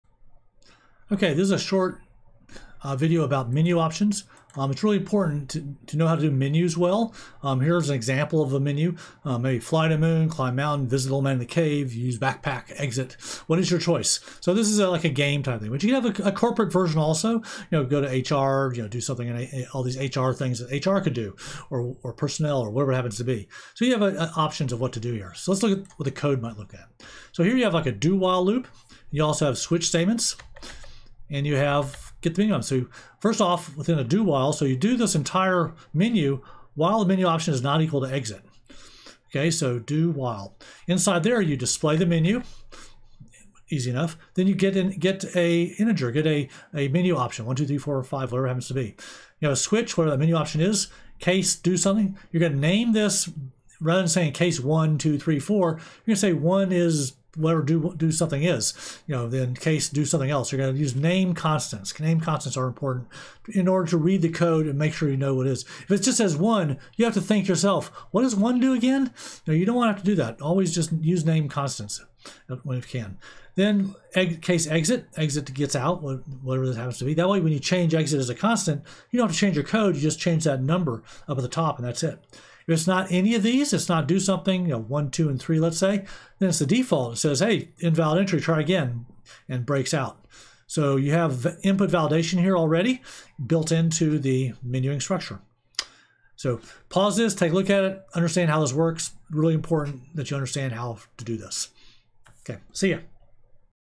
CIS129: Using Menu Options
A Do-While loop with a Switch-case structure inside.